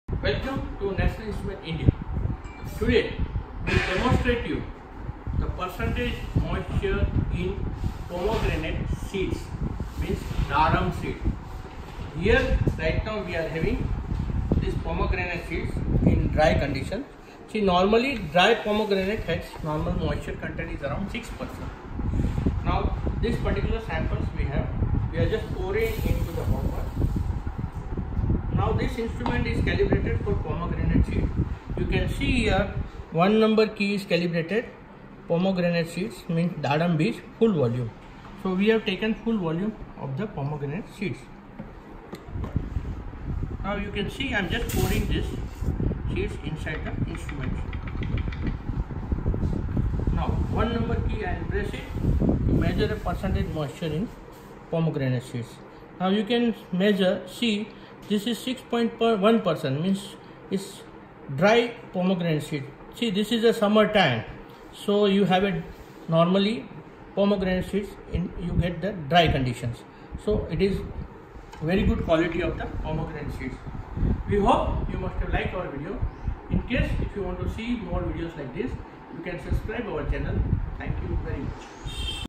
Pomegranate Seed- अनार बीज -દાડમના બીયા Digital Moisture Meter
Measurement of % Moisture in Pomegranate seed

National Marketing
#7,8,9 Gunatit Estate,
Opp Atladara railway station,
Ph: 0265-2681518  , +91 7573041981, 2, 3, 4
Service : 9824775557